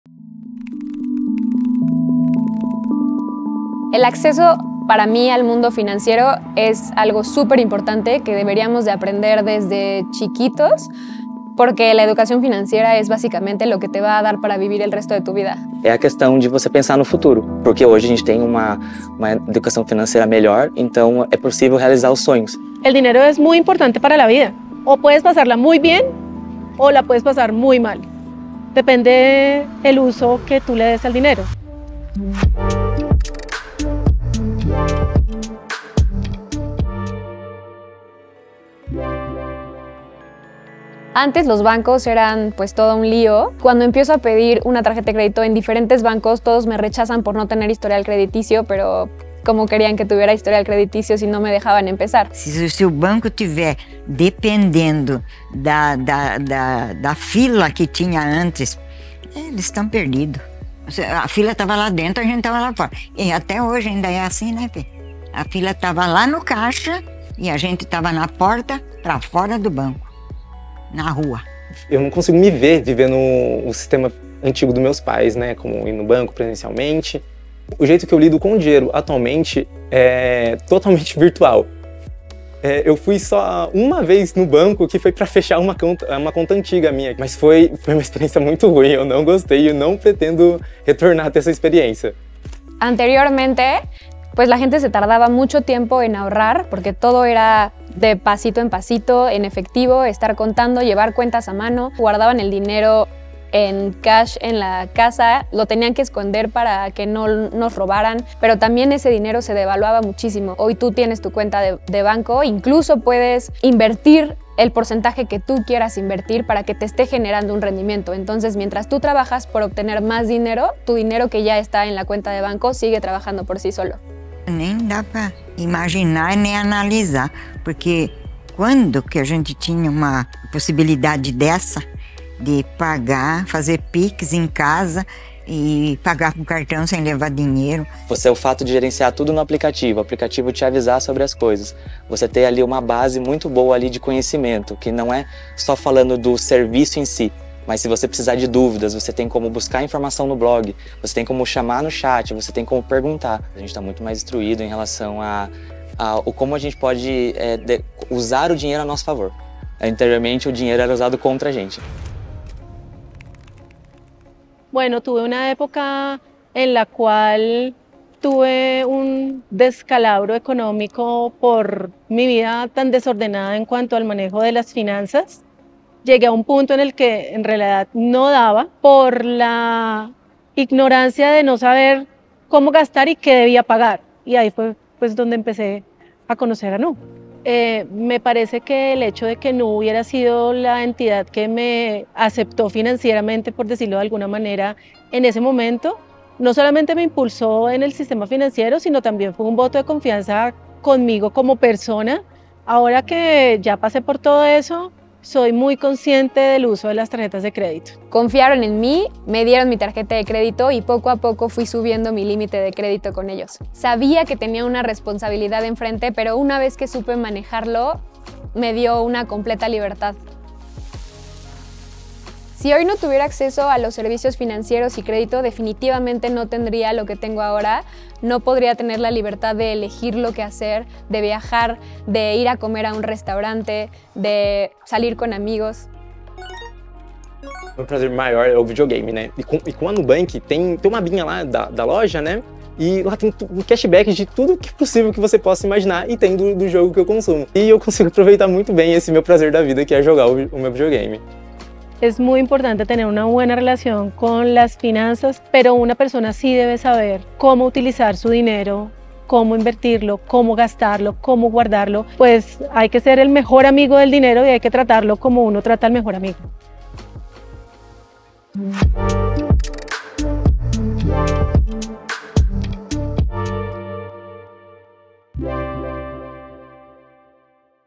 A nova relação com o dinheiro | La nueva relación con el dinero | The new relationship with money
Através do olhar de clientes do Nu, acompanhe a transformação na relação das pessoas com o dinheiro em três das maiores economias da América Latina: Brasil, México e Colômbia. Por meio de seus depoimentos, os clientes comentam sobre como a migração para o universo digital não apenas facilitou suas vidas financeiras, mas também abriu portas para além da conveniência, proporcionando educação financeira acessível e os introduzindo ao mundo dos investimentos.

A través de la perspectiva de los clientes de Nu, acompaña la transformación en la relación de las personas con el dinero en tres de las mayores economías de América Latina: Brasil, México y Colombia. Mediante sus testimonios, los clientes comentan cómo la migración al universo digital no solo facilitó sus vidas financieras, sino que también abrió puertas más allá de la conveniencia, proporcionando educación financiera accesible e introduciéndolos al mundo de las inversiones.

Through the eyes of Nu customers, follow the transformation in people's relationship with money in three of the largest economies in Latin America: Brazil, Mexico, and Colombia. Through their testimonials, customers discuss how migrating to the digital world made their financial lives easier and opened doors beyond convenience, providing accessible financial education and introducing them to the world of investments.